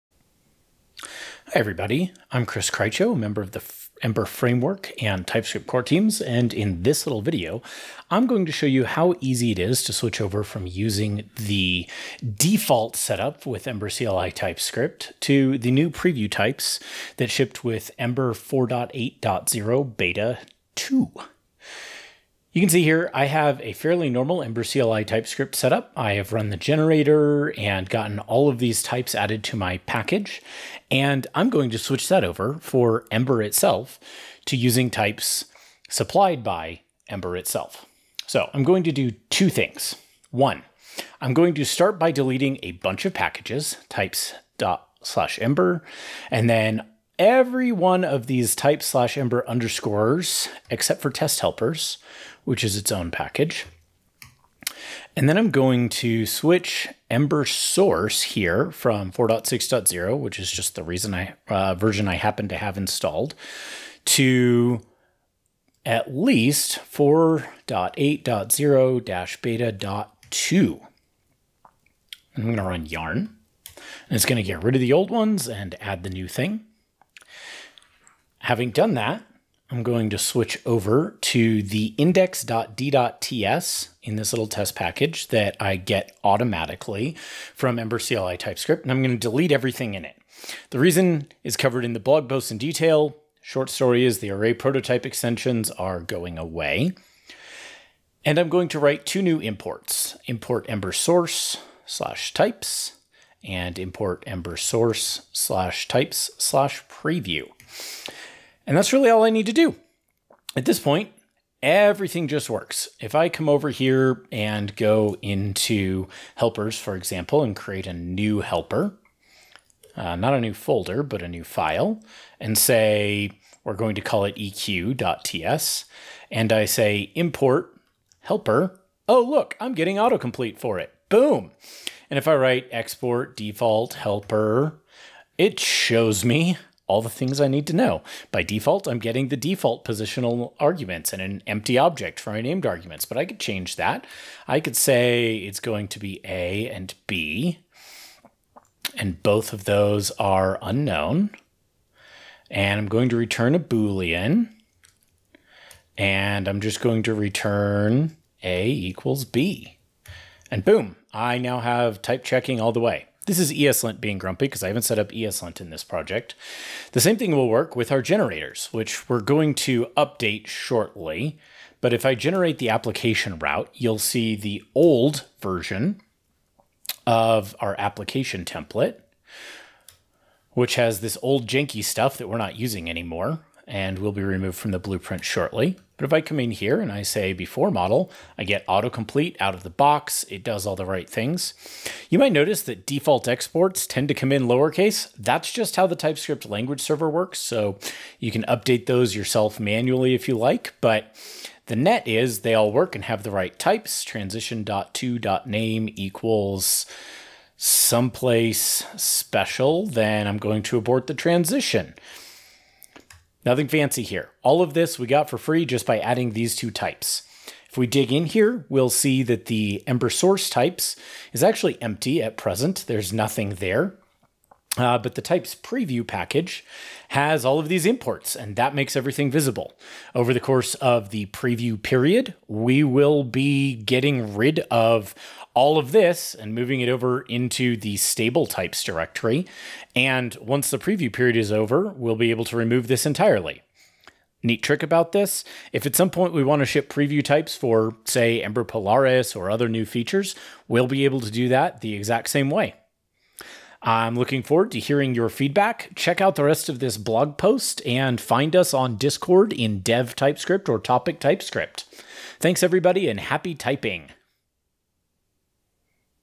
Ember TypeScript Preview Types demo
A screencast showing the transition from types from DefinitelyTyped to preview types shipped from Ember itself.